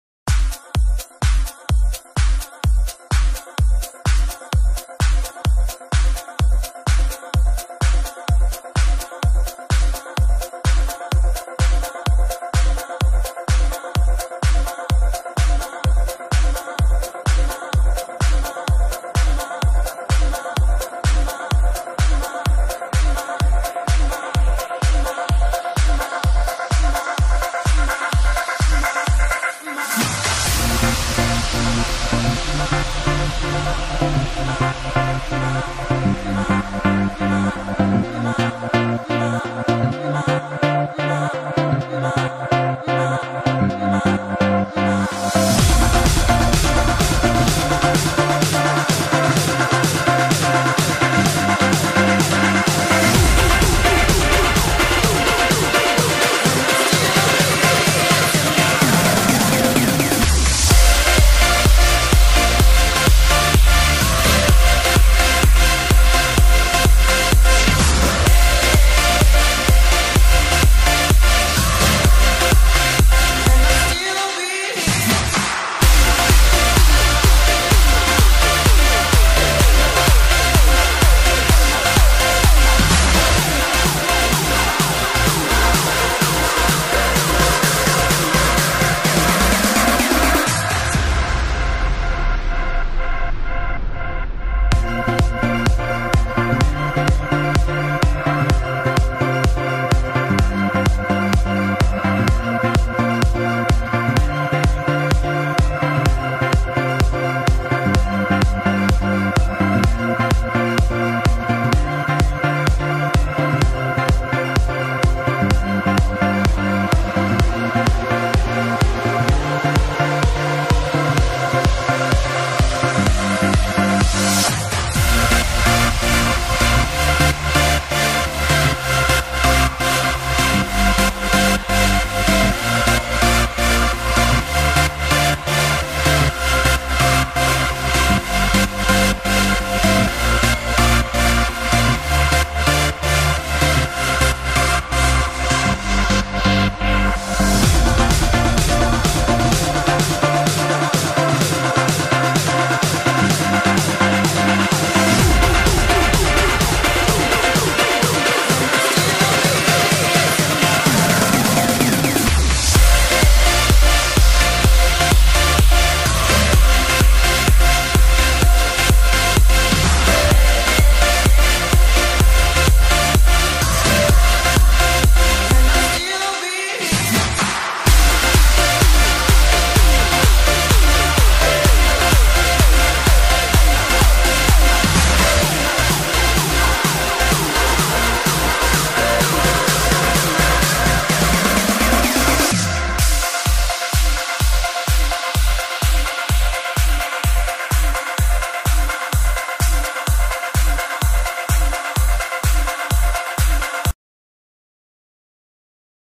Retratos de la Plaza Colón y el Centro de Mar del Plata - Prov. de Bs.As.
Secuencia de fotos del centro de Mar del Plata - Prov. de Bs.As.